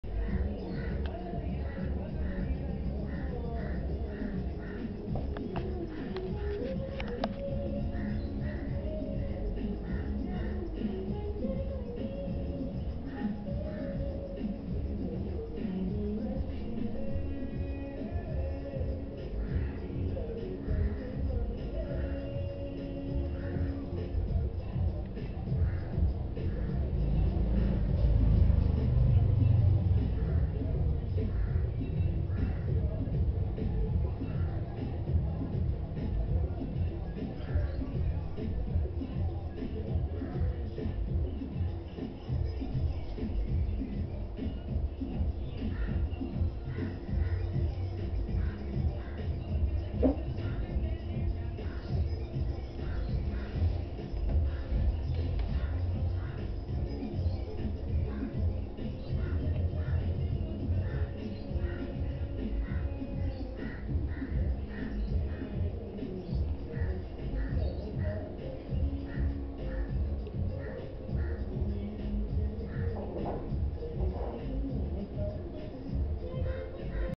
Cat enjoying catnip root
Cat's like this roots a lot.they eat and rub it all over the body too